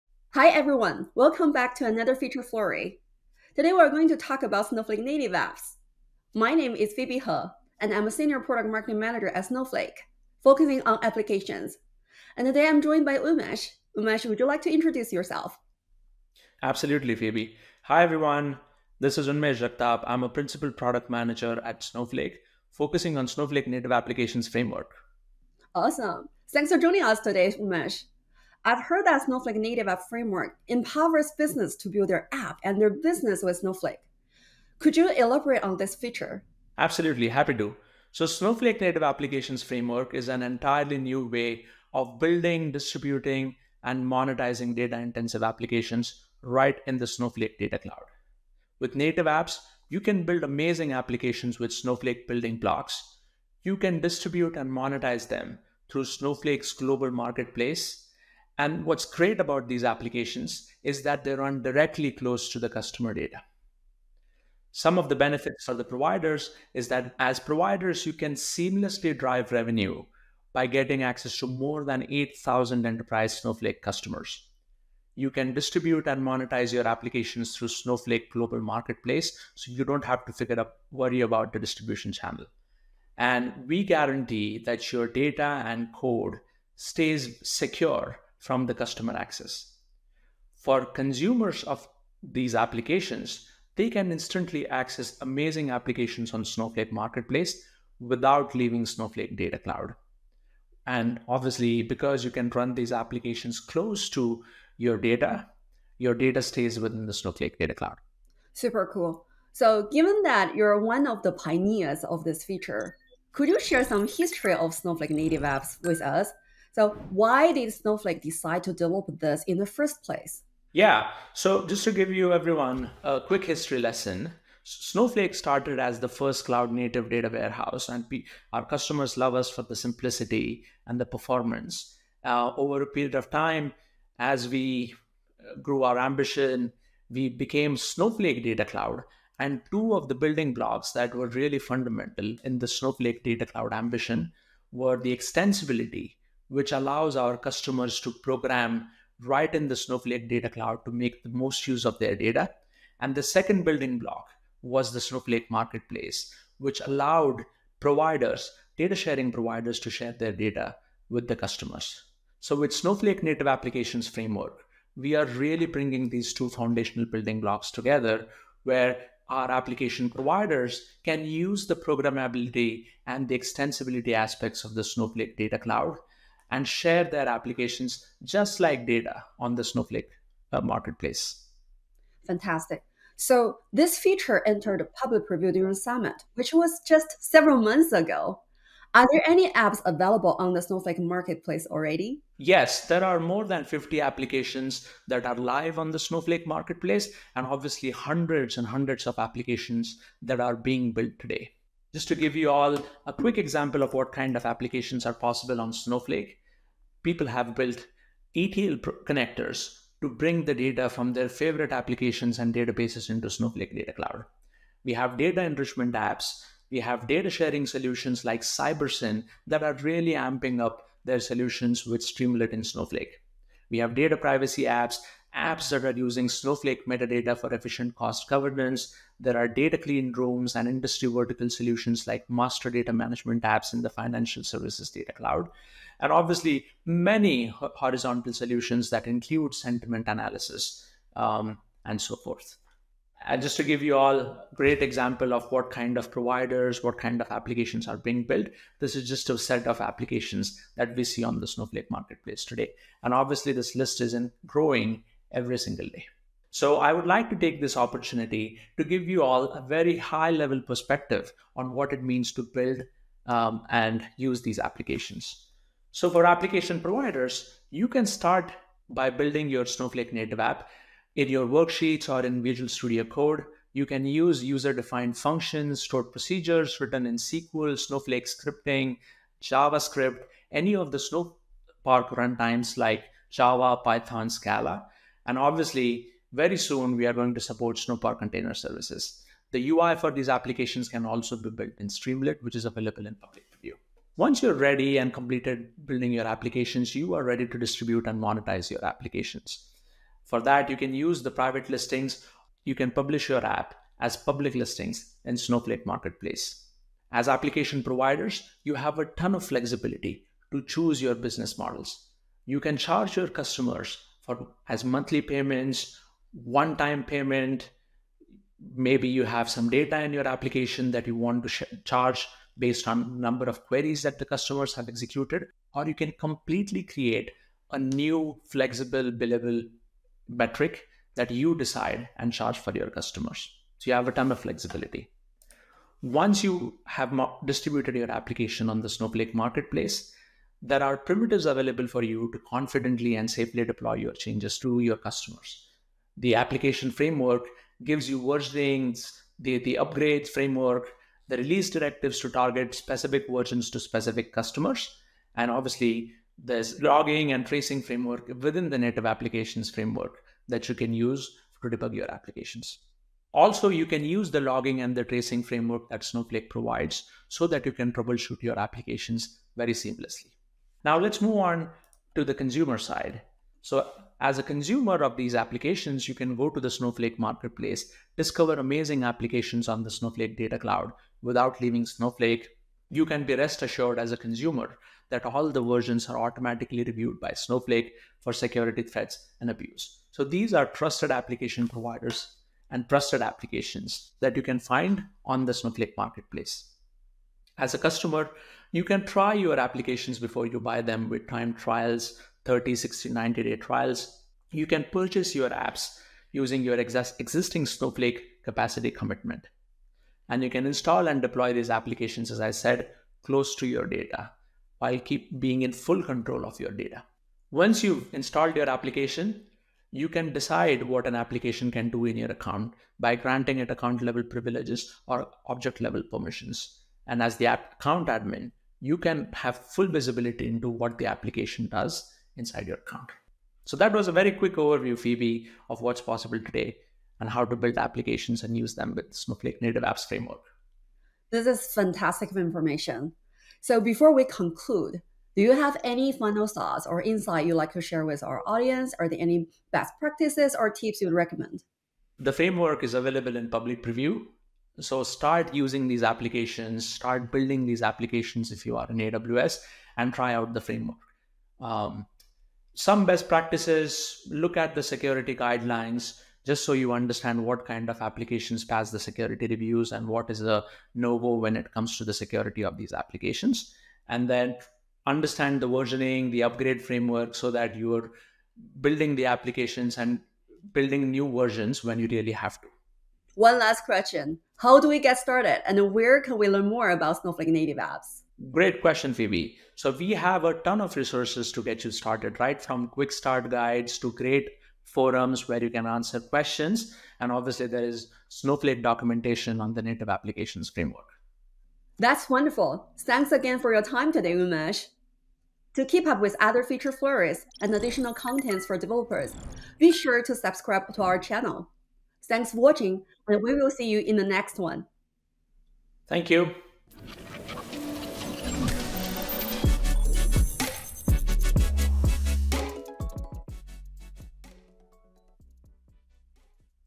Feature Flurry: Build Your App And Your Business With The Snowflake Native App Framework
Leverage the Snowflake Native App Framework to build an app and market, monetize, and distribute it to customers across the Snowflake Data Cloud seamlessly and securely.